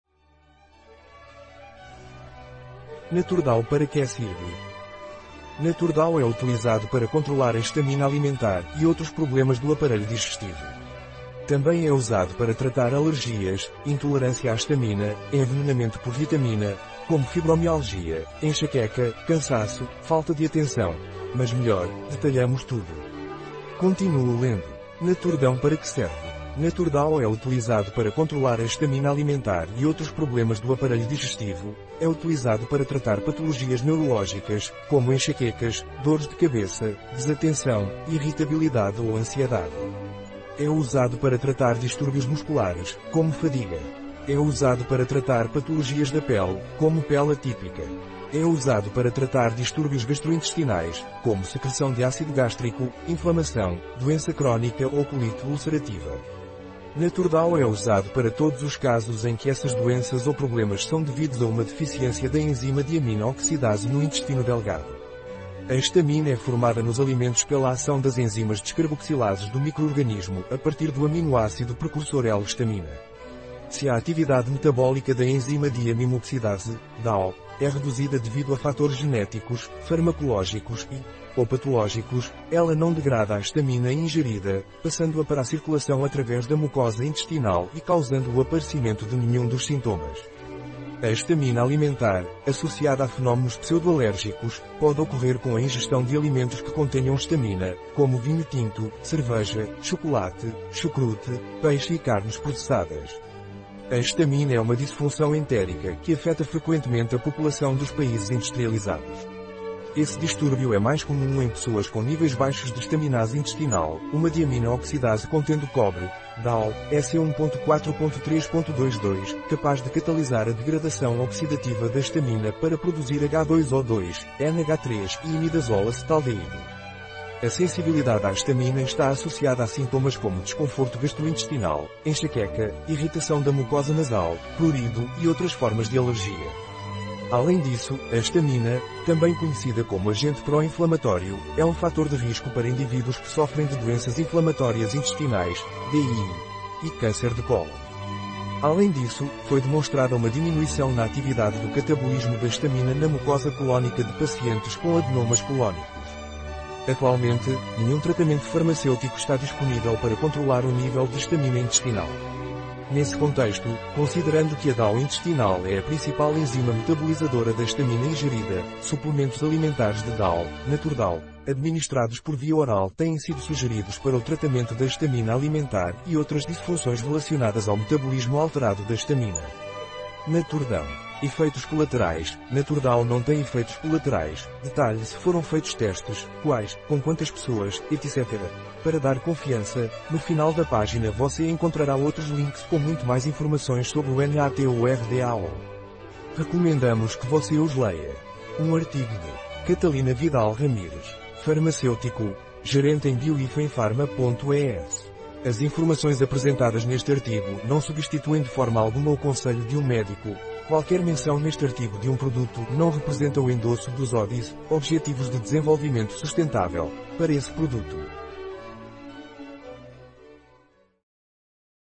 NATURDÃO Para que serve?
NATURDAO é utilizado no controle da histaminose alimentar e outros problemas do aparelho digestivo. Também é usado no tratamento de alergias, intolerância à histamina, envenenamento por hitamina; como fibromialgia, enxaqueca, cansaço, falta de atenção... mas melhor, detalhamos tudo para você. Continue lendo NATURDÃO Para que serve?